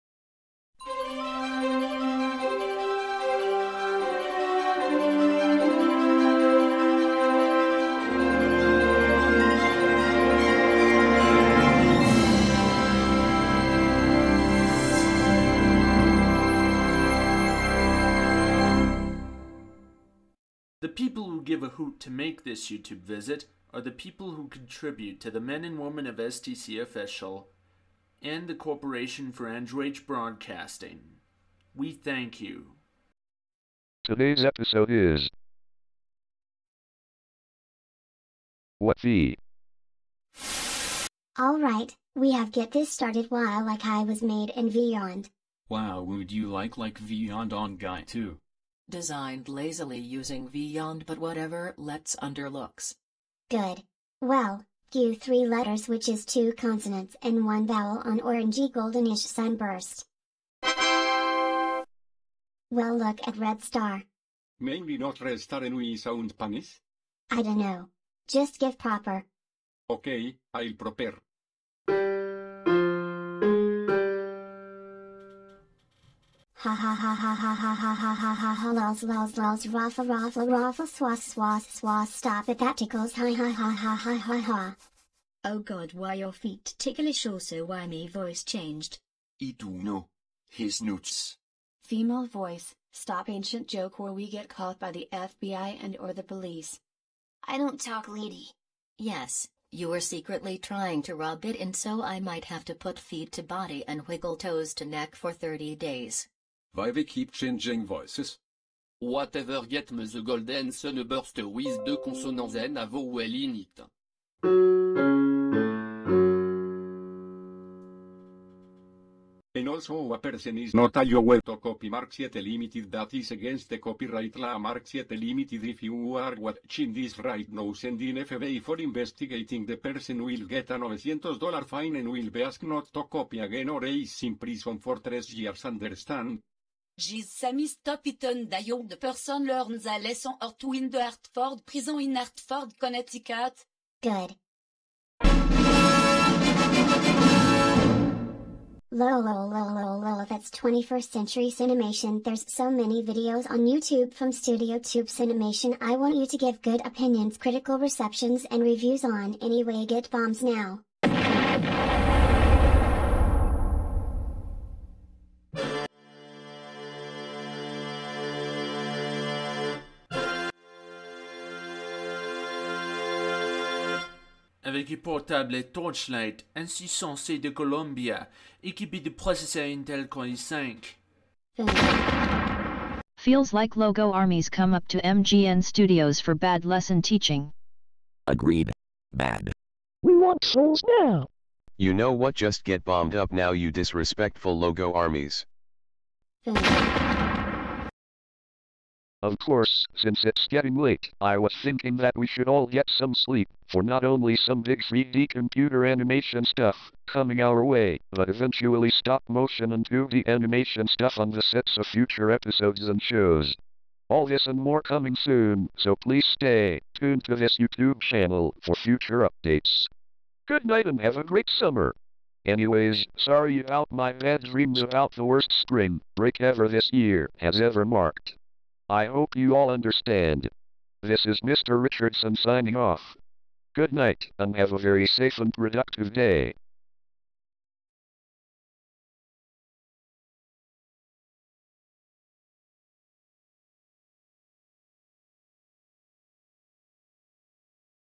[#1956] CET Connect Logo Bloopers | Episode 19 | WHAT THE...? | STC Official / STC Officiel
[Based on a Monsters Inc. parody entitled "Gamers Inc."]

BLOOPERS INCLUDED:
- MGN Red Star (2017)
- STC (2020-present)
- Mark VII Limited (1969)
- 21st Century Cinemation's Print Logo (B&W Variant)
- Armies of StudioTubeCinemation and FM.com Inc. Logos (a 2-in-1 Army!)